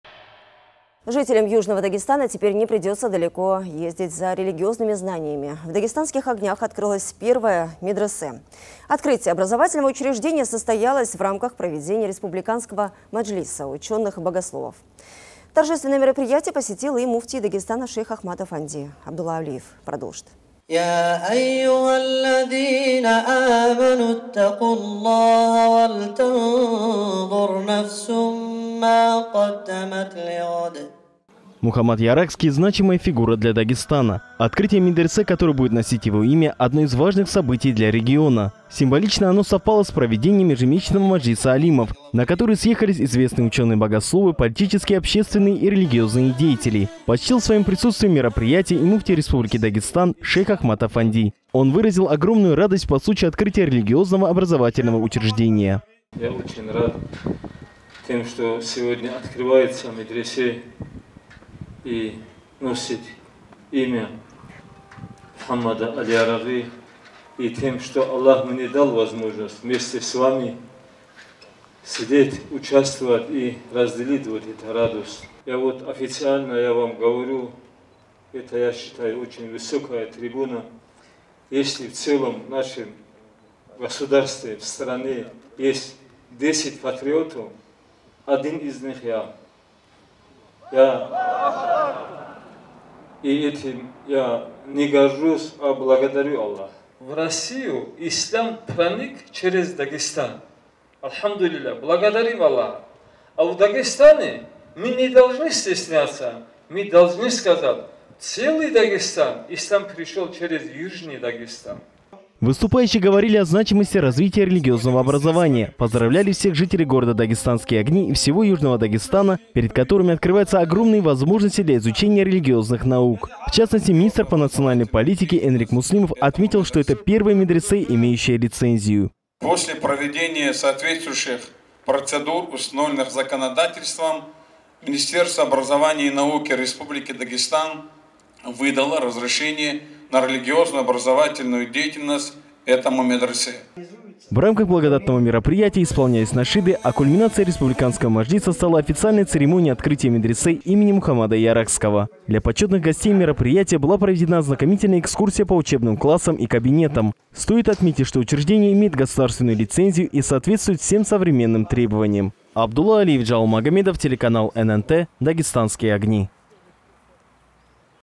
Первое лицензированное медресе в южном Дагестане
Жителям Южного Дагестана теперь не придется далеко ехать за религиозными знаниями. В городе Дагестанские огни открылось первое лицензированное медресе. Открытие образовательного учреждения состоялось в рамках проведения республиканского маджлиса ученых-богословов. Торжественное мероприятие посетил Муфтий Дагестана шейх Ахмад-Афанди